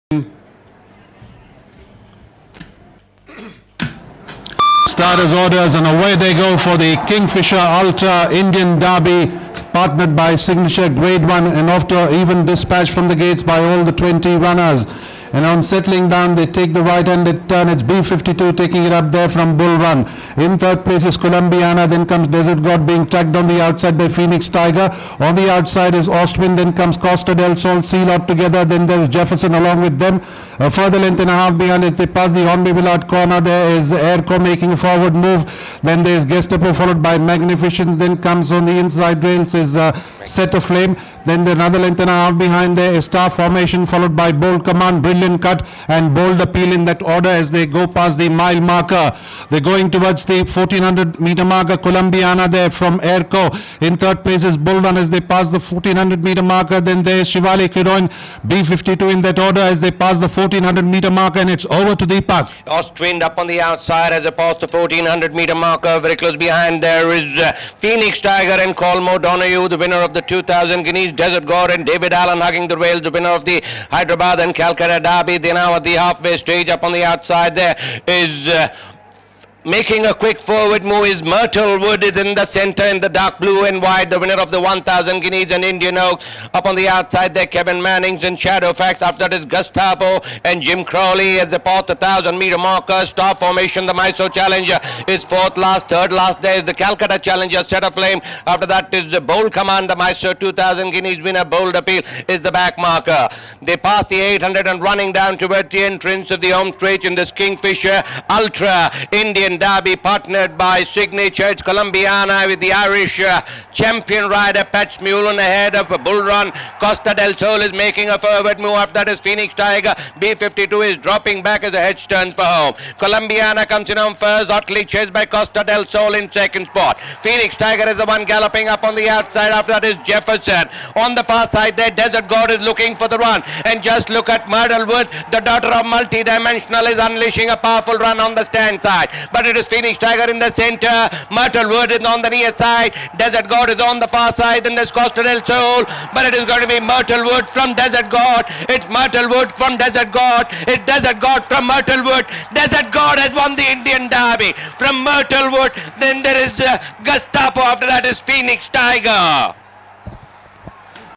Desert God ridden by David Allan wins The Indian Derby 2016
Desert God ridden by David Allan won the Kingfisher Ultra Indian Derby (Gr. I) in a thrilling finish from Myrtlewood at the races here on Sunday (Feb. 7). The winner is owned by Mrs. Sharmila Padmanabhan and Mr. S.Padmanabhan rep. Radiant Bloodstock Pvt Ltd & Col. Swapan Bhadra. S.Padmanabhan trains the winner.

KINGFISHER ULTRA INDIAN DERBY (Gr. I) partnered by Signature (2,400m), 4-y-o only:

DESERT GOD (BURDEN OF PROOF — RUNNING FLAME) (DAVID ALLAN) 1,

MYRTLEWOOD (MULTIDIMENSIONAL — ADAMILE) (P.S.CHOUHAN) 2,

GESTAPO (MASTERCRAFTSMAN — DAJOLIE) (JIM CROWLEY) 3 and

PHOENIX TIGER (PHOENIX TOWER — TIGRESSE NOIRE) (DONOGHUE) 4.

1/2, 1-1/2, 1-1/4. 2m 29.36s. Rs. 72 (w), 22, 14 and 100 (p). SHP: Rs. 31, FP: Rs. 117, Q: Rs. 129, Tanala: Rs. 4, 586 and Rs. 2,620.

Favourite: Myrtlewood. Owners: Mrs. Sharmila Padmanabhan and Mr. S.Padmanabhan rep. Radiant Bloodstock Pvt Ltd & Col. Swapan Bhadra . Trainer: S.Padmanabhan.